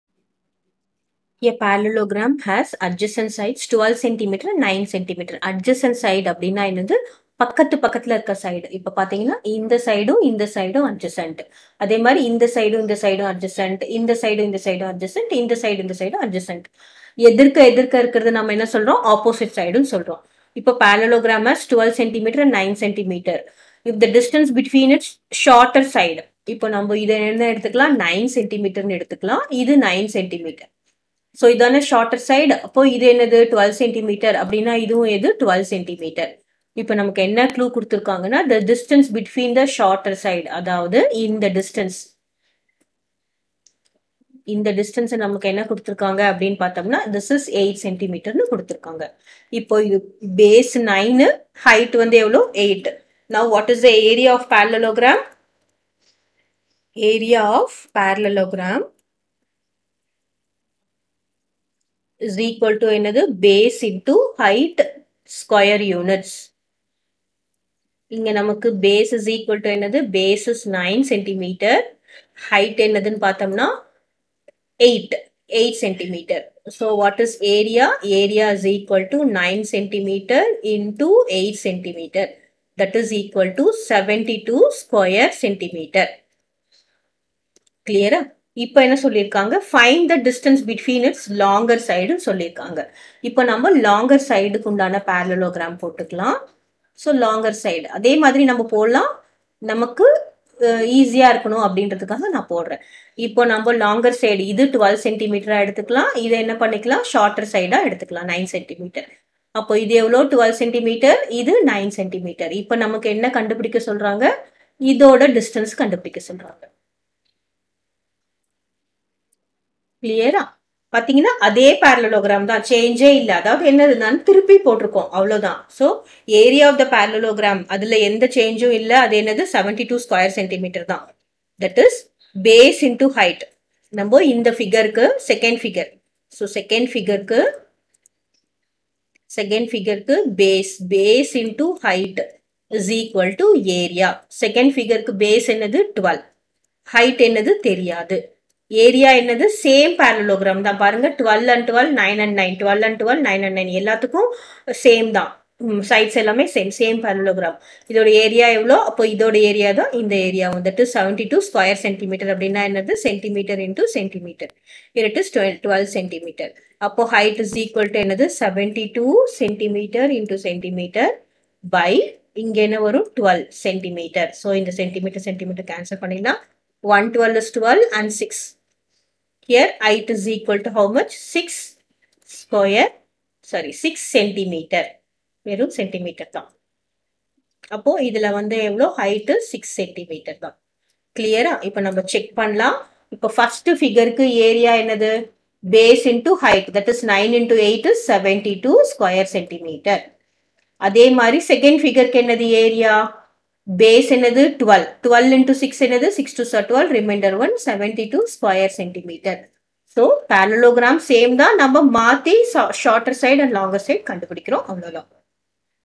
Class 7 TN Maths Term I 2.MEASUREMENTS Example 2.4 A parallelogram has adjacent sides 12 cm 9 cm
Term I  2.MEASUREMENTS
Example 2.4
A parallelogram has adjacent sides 12 cm and 9 cm. If the distance between its shorter sides is 8 cm, find the distance between its longer side